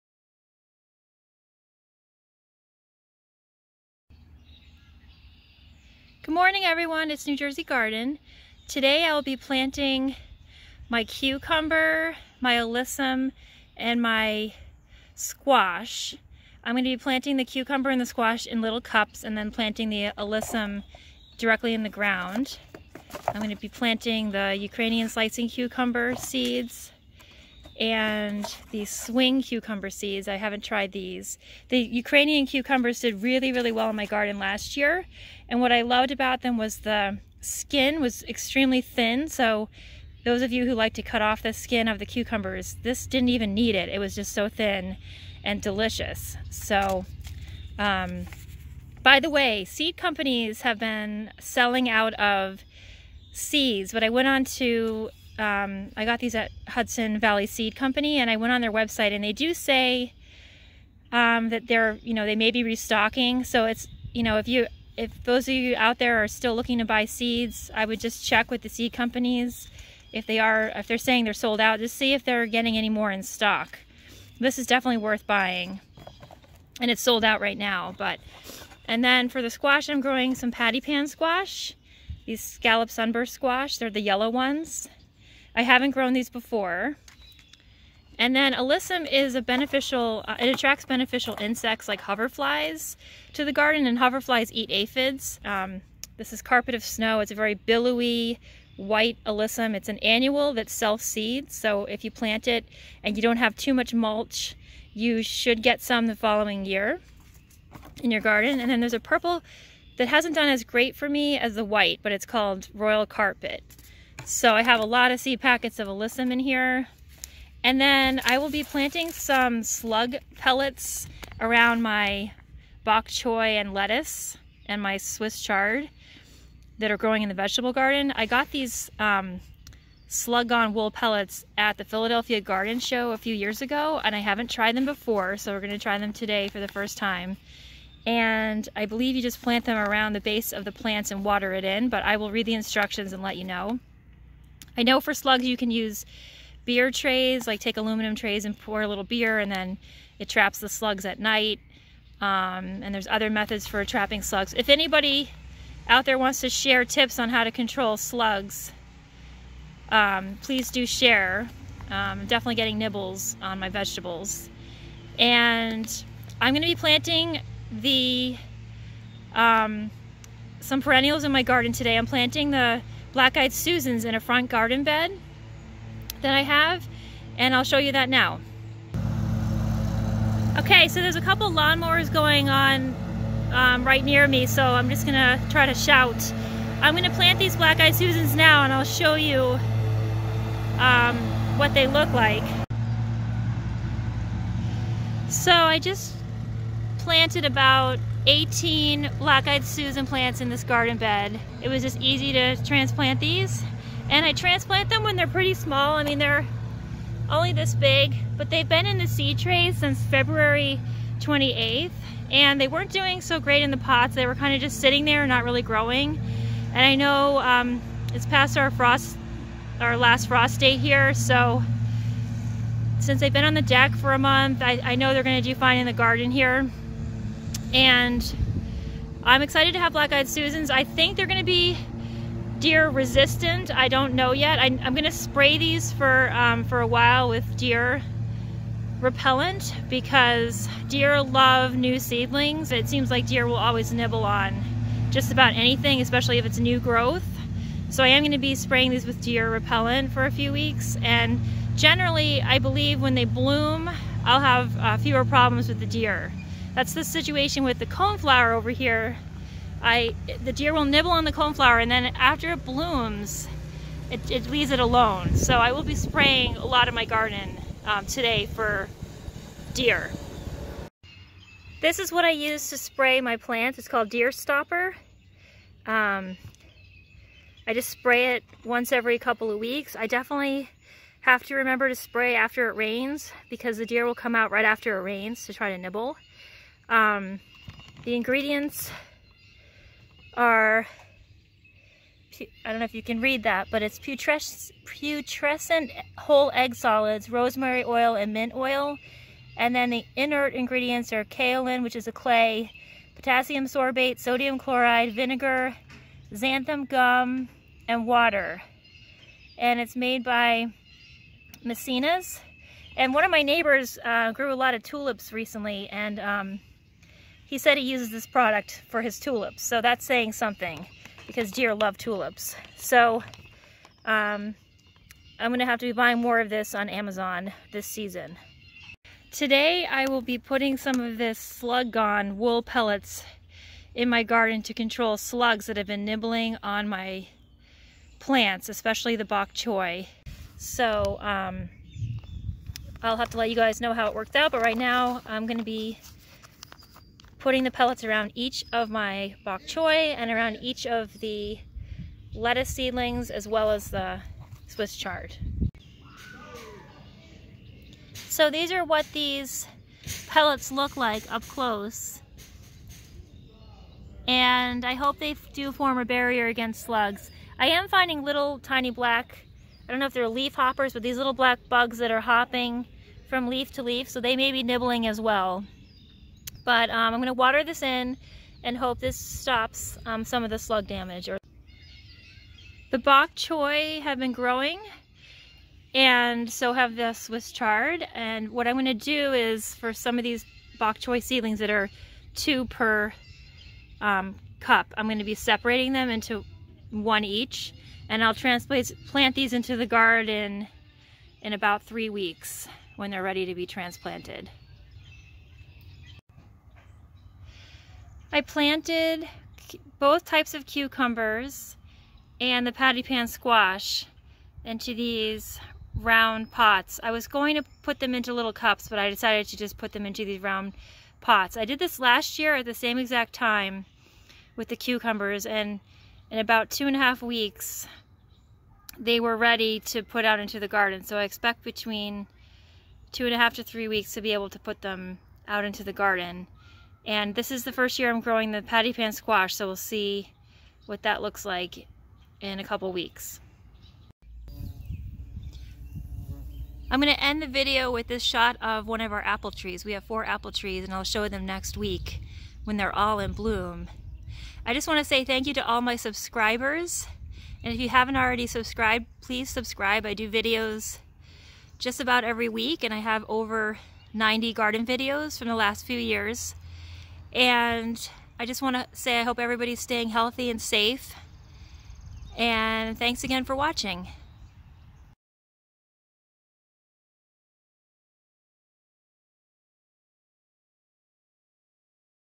May Plantings and Slug Control
This weekend I planted my Black-Eyed Susan seedlings into the ground and I added wool slug repellant to my swiss chard, bok choy, and lettuce seedlings in the vegetable garden.  Also, I was very excited to plant my cucumber and patty pan squash seeds into containers. I will moving the cucumber and patty pan squash seedlings into the vegetable garden in about 2-3 weeks.